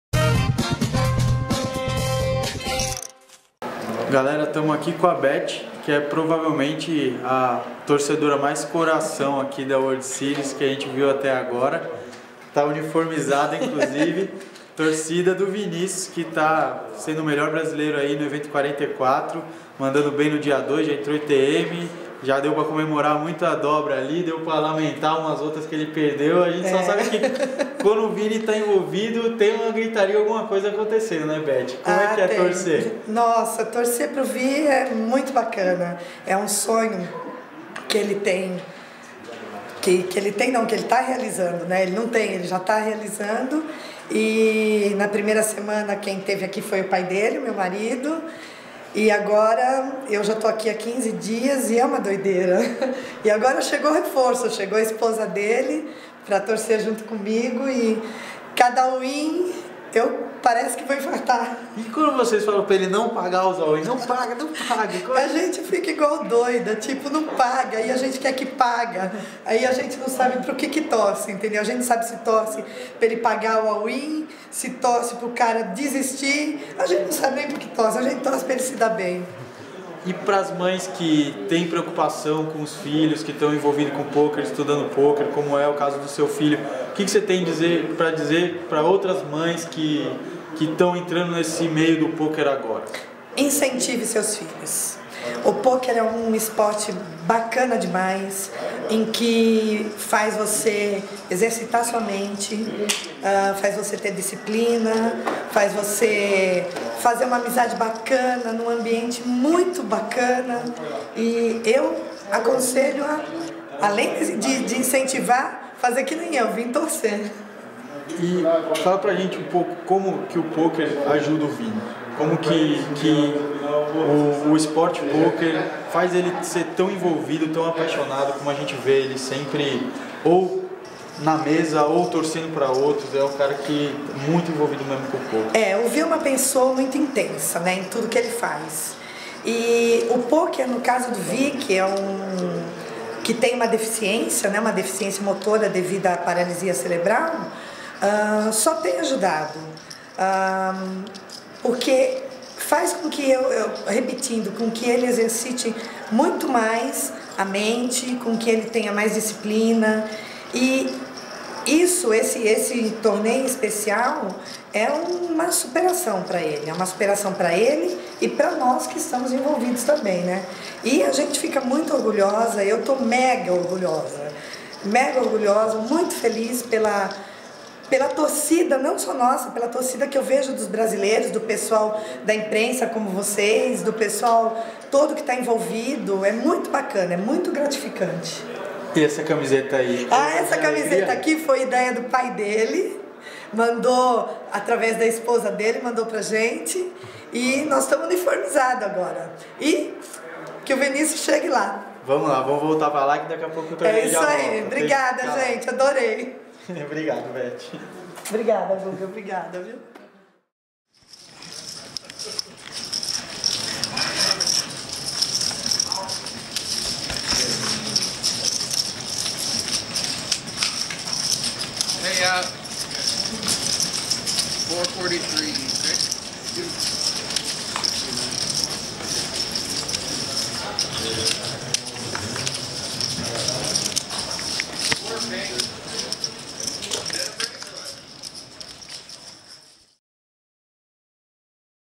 Beth Silva, torcedora número 1 da WSOP
Beth Silva é mãe de Vinicius, um jogador de poker paulista que teve paralisia cerebral. No dia em que o filho fez bonito no Evento 44, ela contou ao Pokerdoc um pouco da história de superação de Vinicius, um orgulho do poker brasileiro.